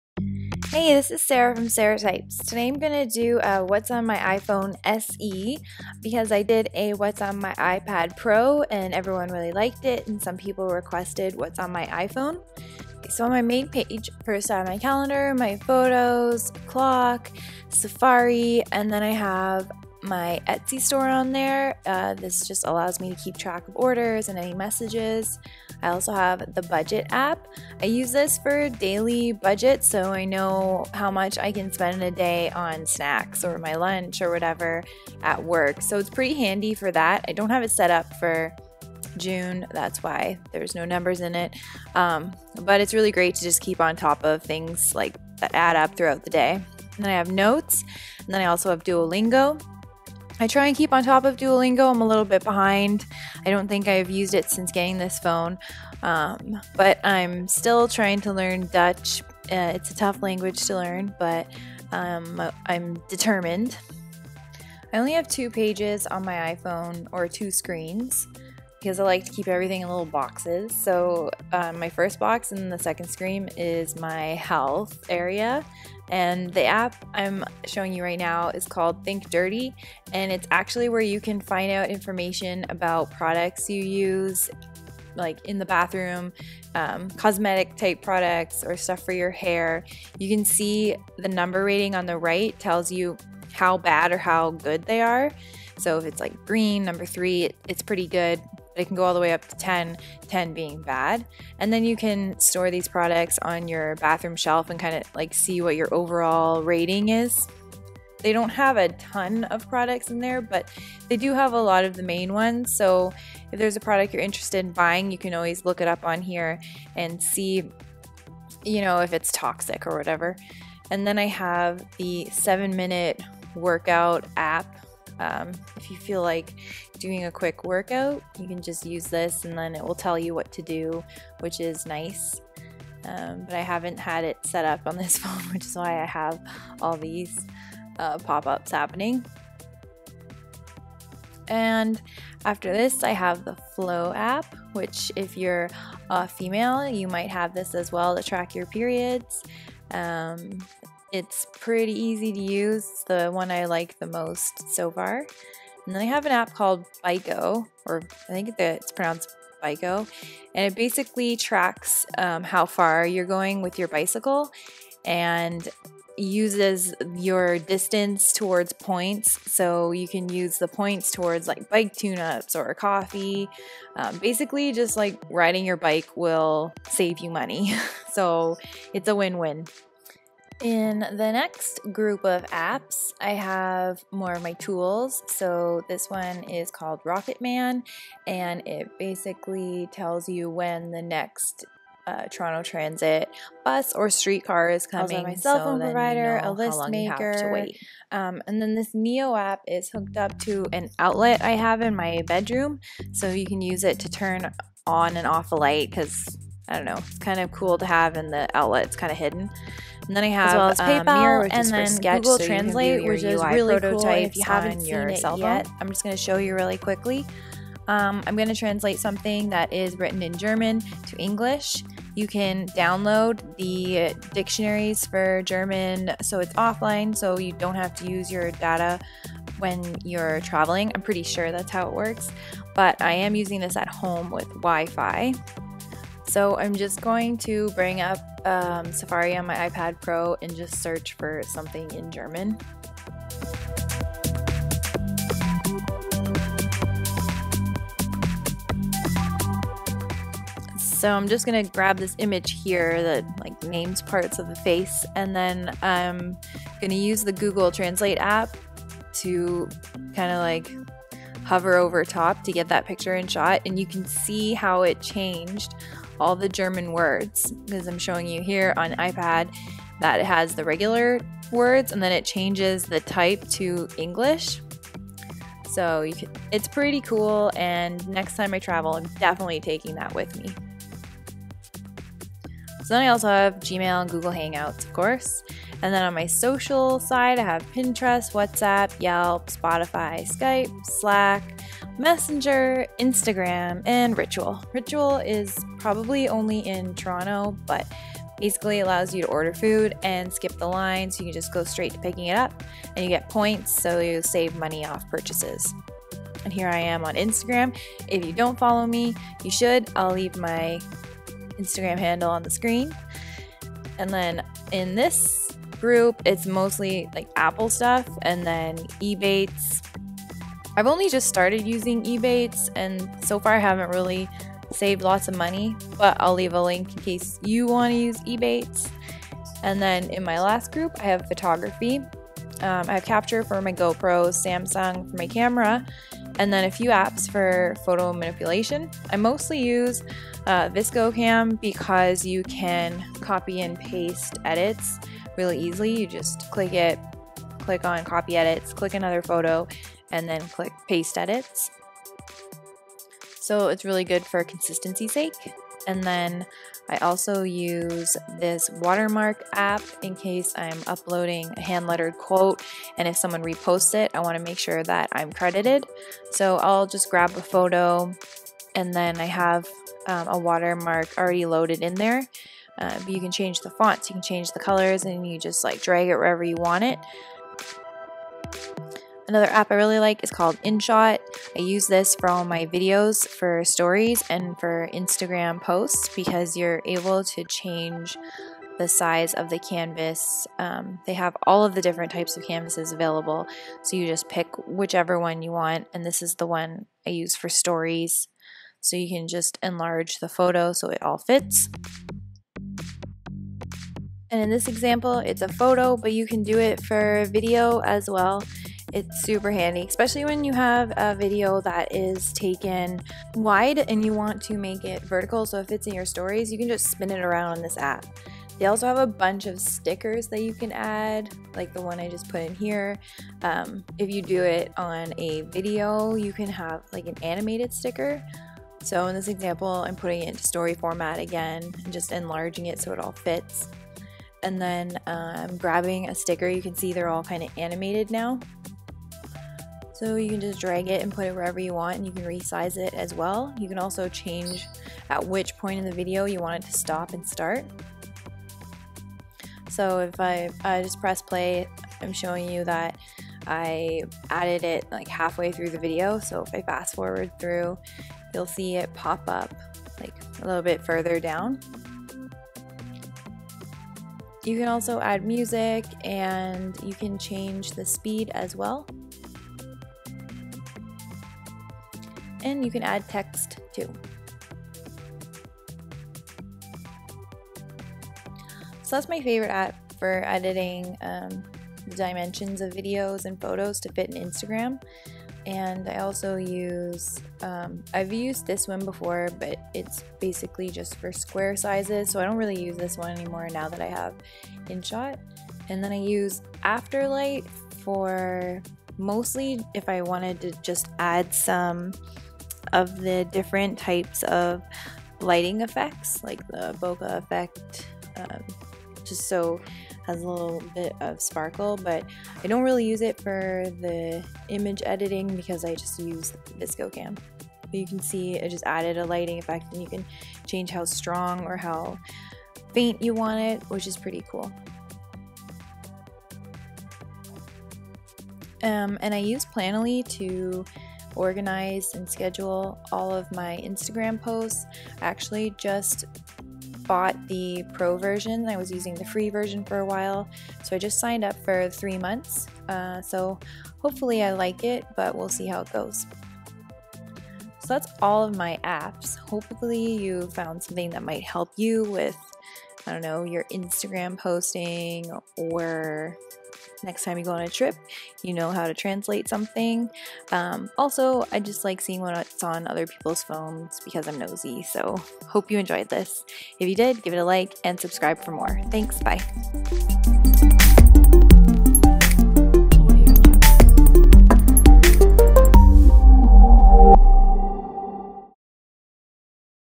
What's on my iphone SE, How I edit my Instagram Photos and Videos
Because I'm nosey and like to see what's on other people's iPhones too! Bonus! I show you my favourite apps for photo and video editing.